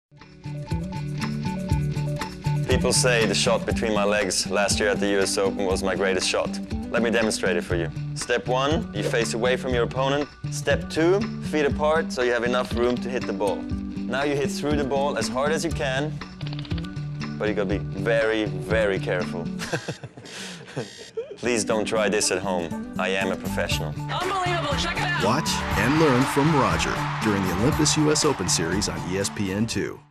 2010 Olympus US Open Series: Roger Federer
Watch Roger illustrate his signature "tweener" shot that helps him conquer the tennis world.

Tune in all summer to the Olympus US Open Series in addition to television coverage on ESPN2 & The Tennis Channel.